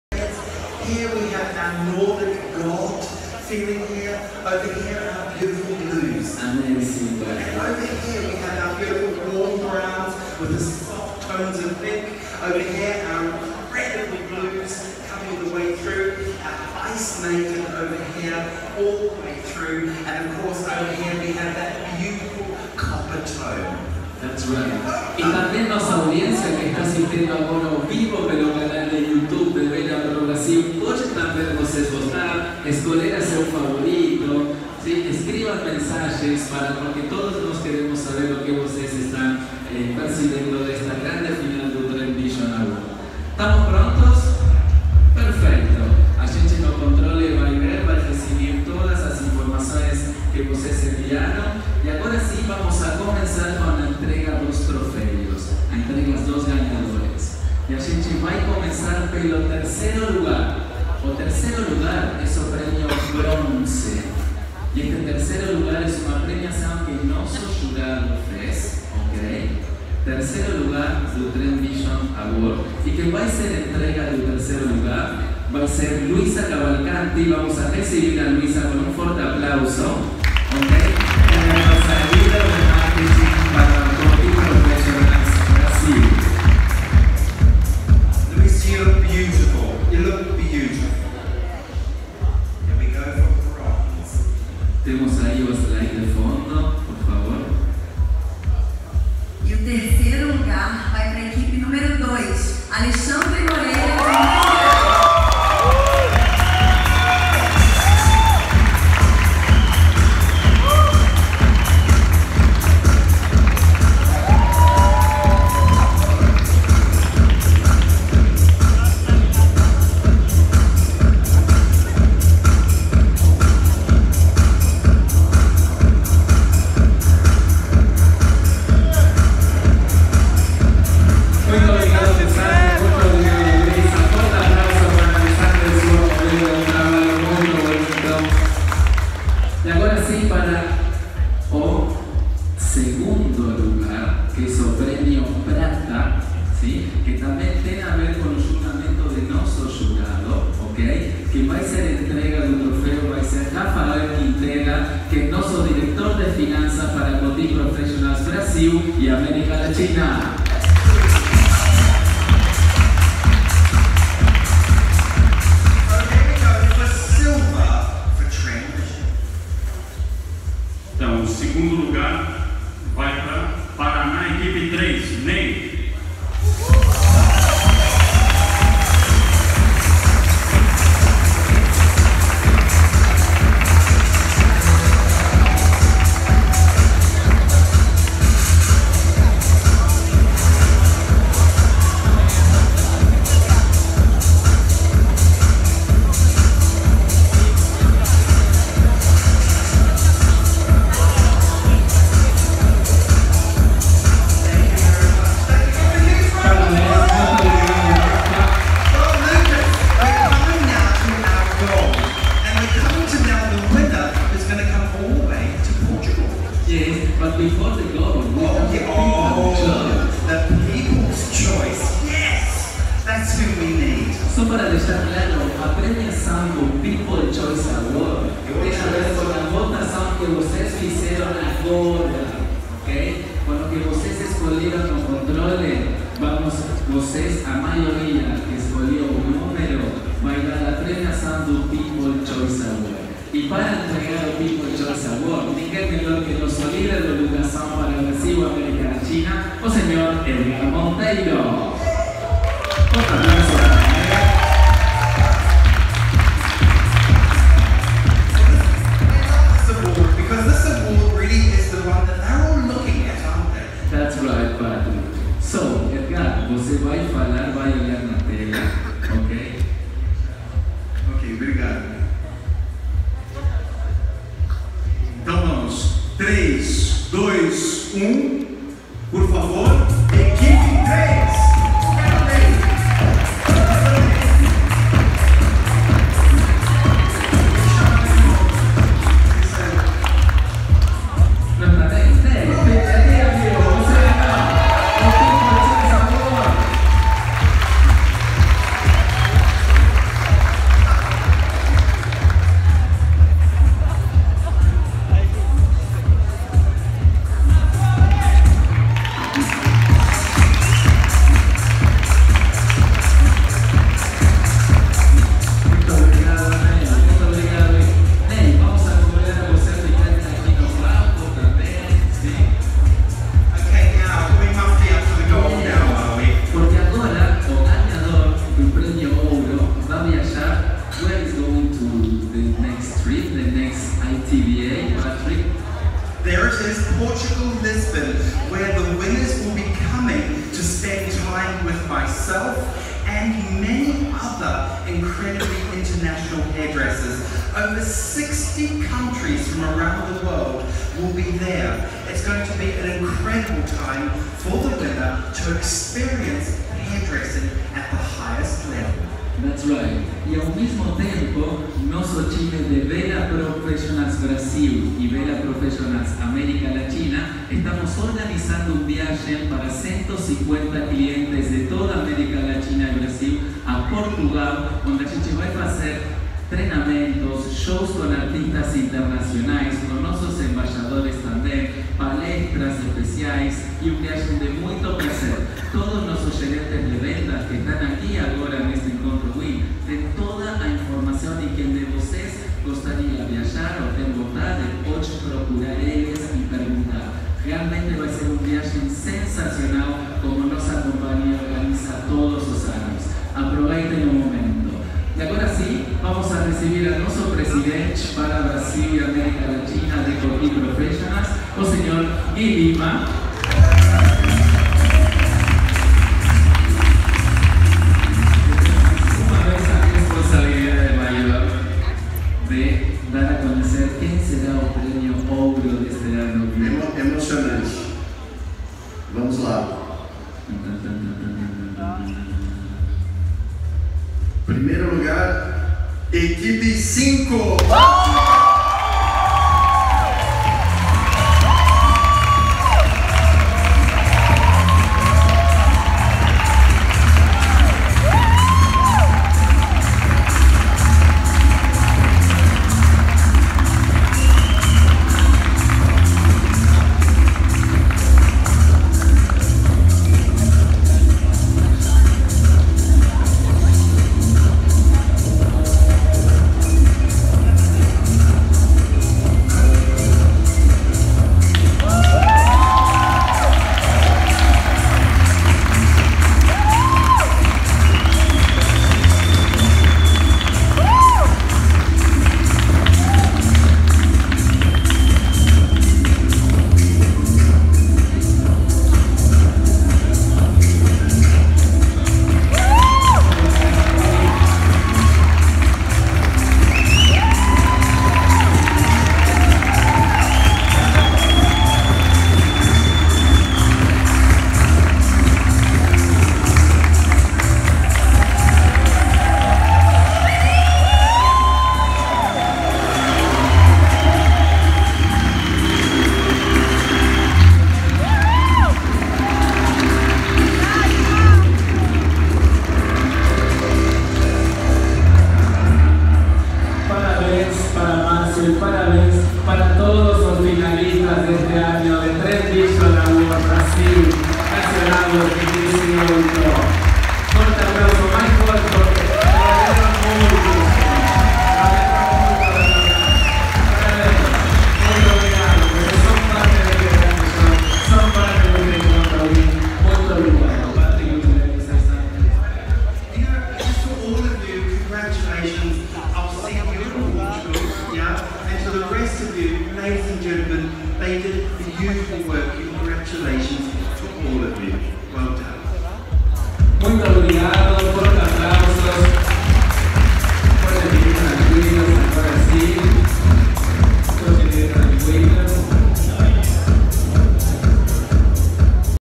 Final do Trend Vision Awards 2018
Veja quem venceu o TVA 2018, da Wella Professionals. Márcio Carvalho, do Rio de Janeiro, foi o grande vencedor e vai concorrer ao prêmio internacional em Lisboa, Portugual.

Siga a Cabelos&cia